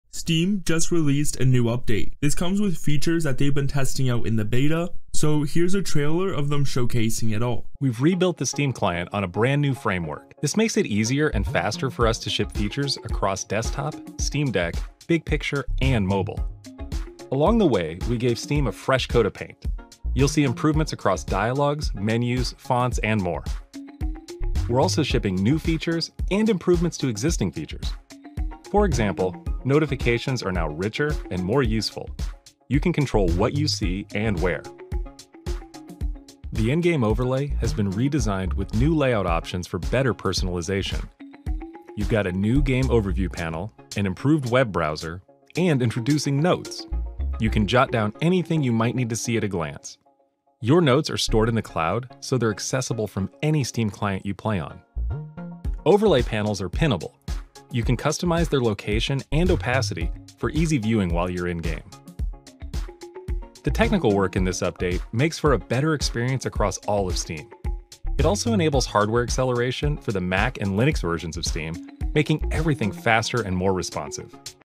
New Trailer For Steam Update
New Steam Update Trailer
new steam desktop app update, steam news, steam update
New features include a completely redesigned in-game overlay, new notes functionality, pinnable in-game windows, improvements to notifications and more.
Steam Desktop Update
Big improvements and new features for the Steam Desktop client are now out of Beta!
New framework, new foundation
The most impactful changes in this update aren’t immediately visible; much of the work went into changing how we share code across the Steam Desktop Client, Big Picture mode, and Steam Deck. These changes also means quicker implementation and iteration of new features. For example, many of the features in this update (like Notes in the overlay) are simultaneously shipping on Steam Deck because of the shared codebase.
Steam gets a fresh coat of paint
This update also brings targeted visual and usability improvements across Steam - we've gone through and updated dialogs, menus, fonts, and colors. The main Steam header and footer, Settings, and the Screenshot Manager are just a few examples of spots with refreshed UI.
And more...
And that's not all! The overlay now includes a Game Overview panel: your one-stop shop to see what’s going on with the game since you last played it. It shows what achievements you have in progress, which friends are also playing, top guides, news, and more. In addition, the browser, achievements, and screenshot manager windows have been updated (both in the overlay and in the desktop client).